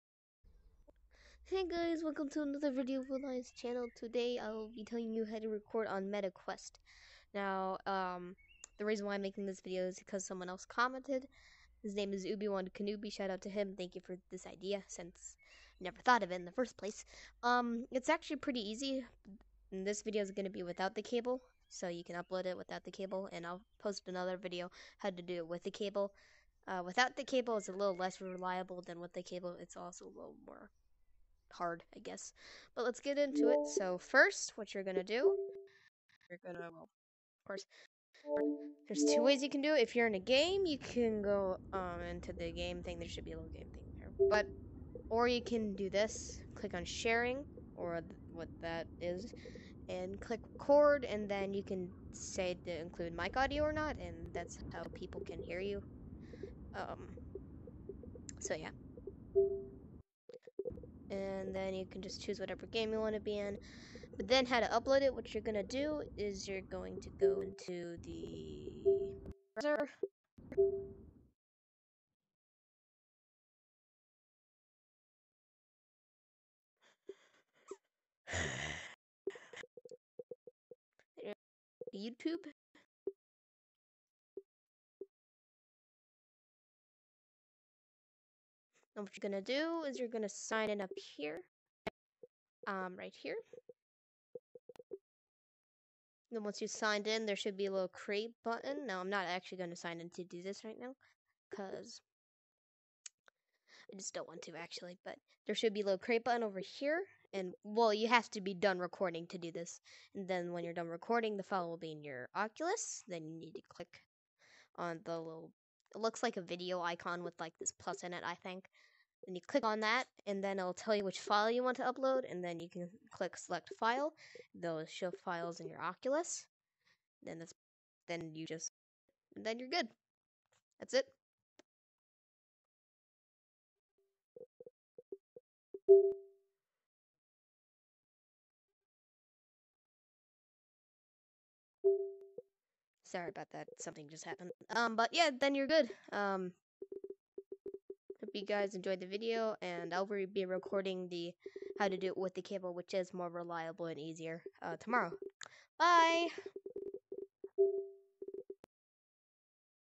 How To Record/Upload (Without Cable) In Meta Quest Vr
This is how to record/upload videos in meta quest. Please leave suggestions for videos that you need help with!